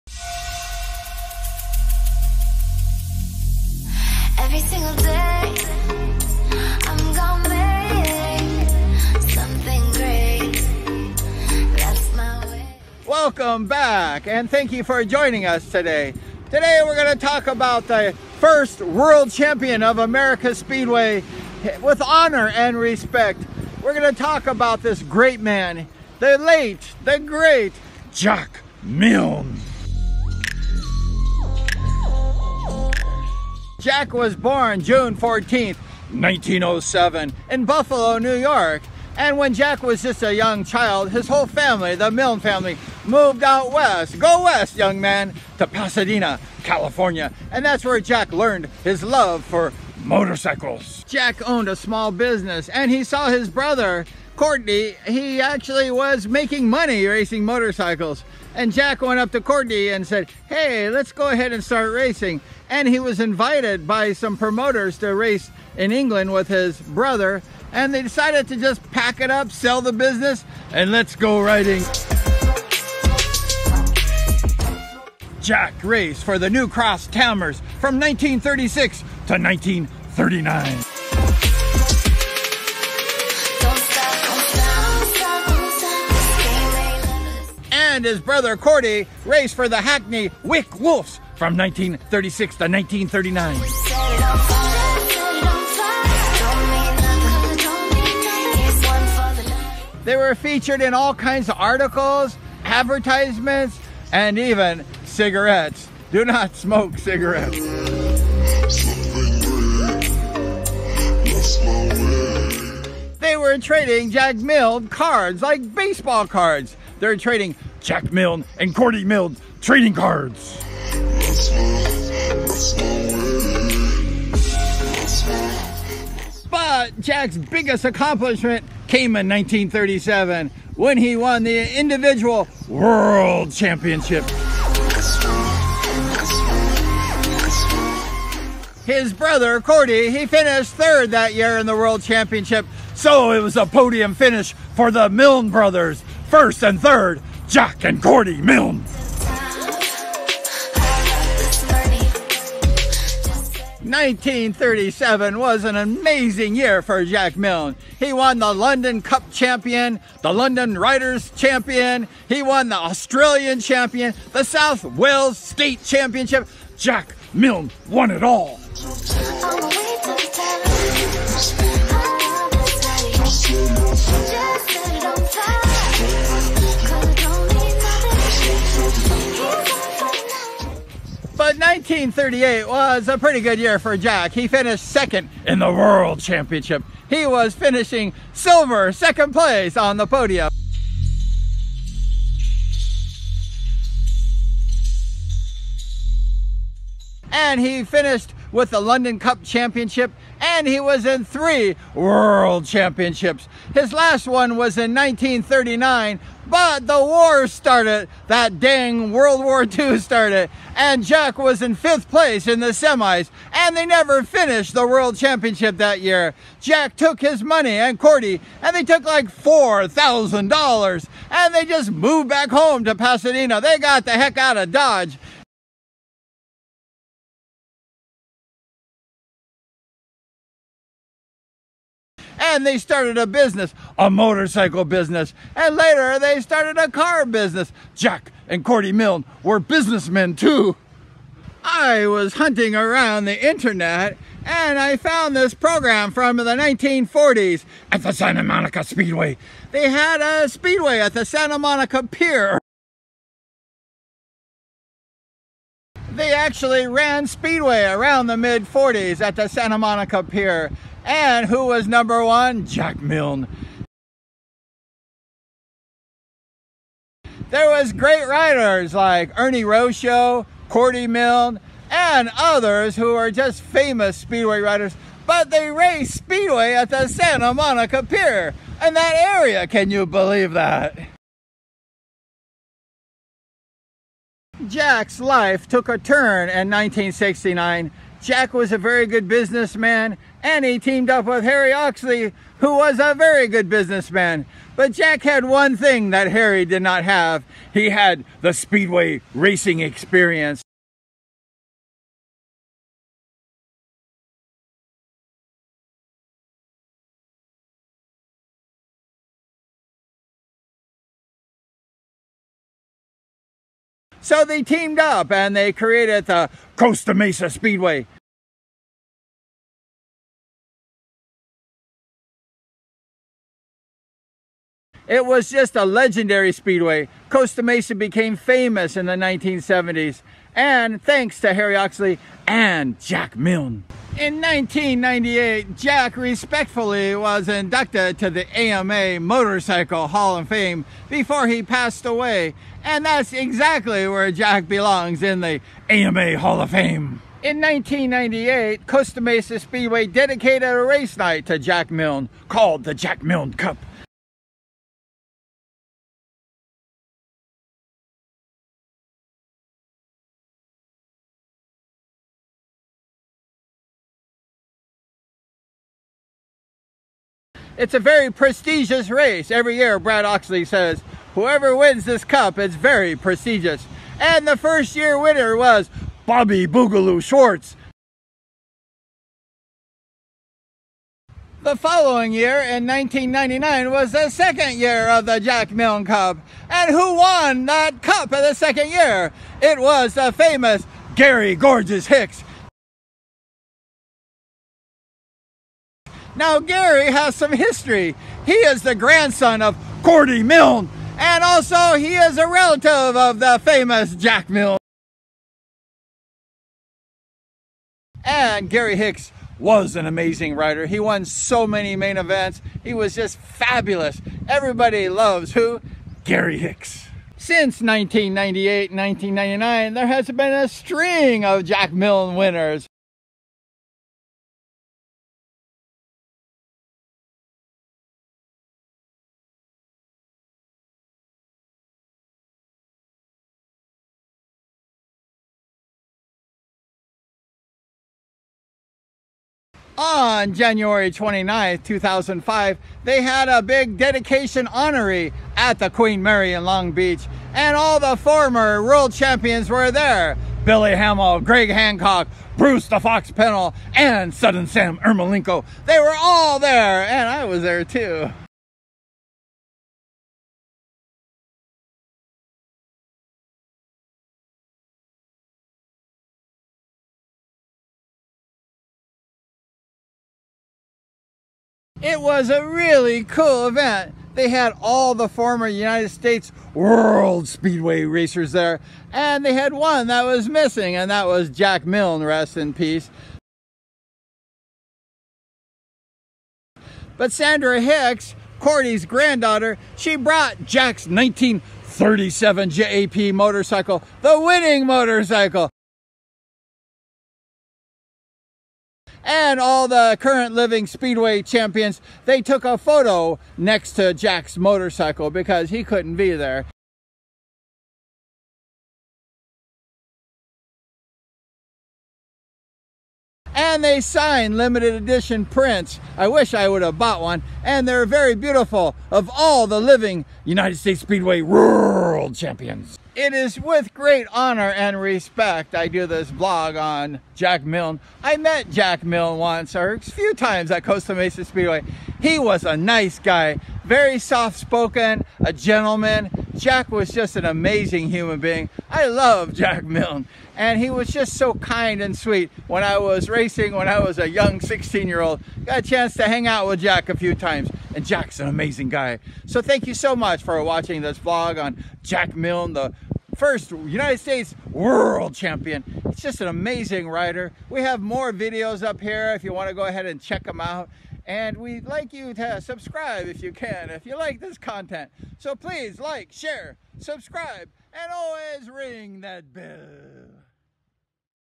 Jack Milne 1937 Speedway World Champion
The First American Speedway World Champion and Jack won his championship only after the second year they started World Championships. He lived in Pasadena, CA and his younger brother Cordy was earning money racing motorcycles and a promoter from England wanted the Milne Brothers in England to race. so Jack sold his small business and the brothers set off a new adventure in England. They became a instant hit and were on ads for all kinds of items, trading cards, and even on packs of cigarettes.

Jack won his World Championship the following year and was on top of the speedway world. In 1938 Jack was runner up to the World Champion and WWII broke out the following year, Jack and Cordy moved back home to Pasadena, CA with $4,000 cash and started a motorcycle dealer shop and later a car dealership in LA.

Jack co-founded the Costa Mesa Speedway with Harry Oxley in 1969 and brought speedway back to America in full force. Costa Mesa Speedway has been running now continually for over 52 years and it world famous to this day!

Jack was an amazing speedway rider and even more amazing human being, Jack Milne Vlog, RIP.